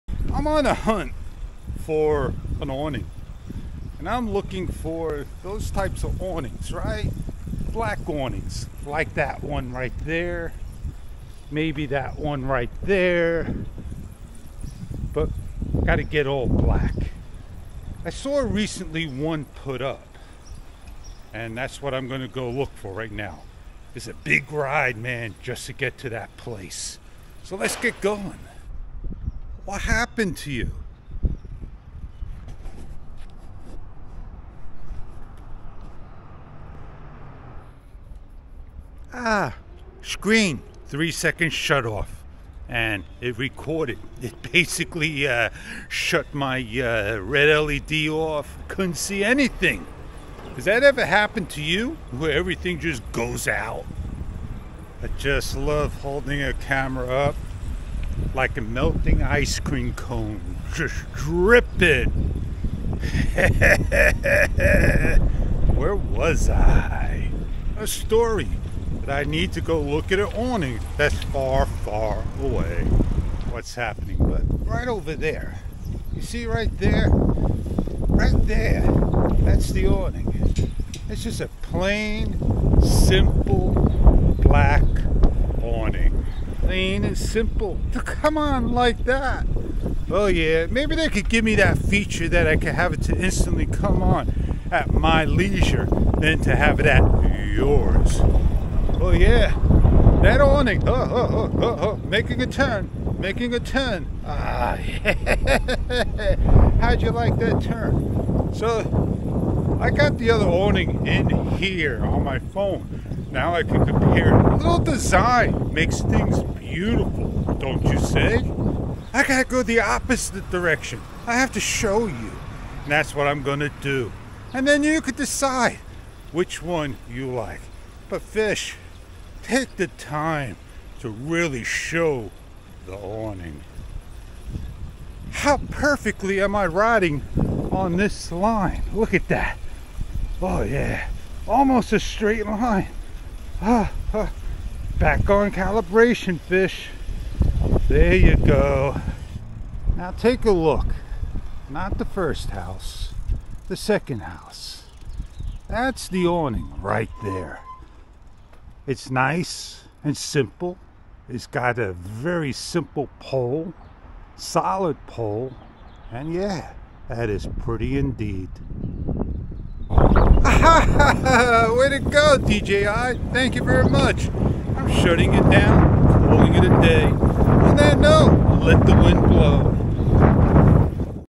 Zorax awning on the search
Going to be   placing a new awning  take the old down, work on the front  and when the new one goes in,  we can sit and look at the  beauty for years to come

□ Electronic Components
□ Hobbies:

- SUPPORT -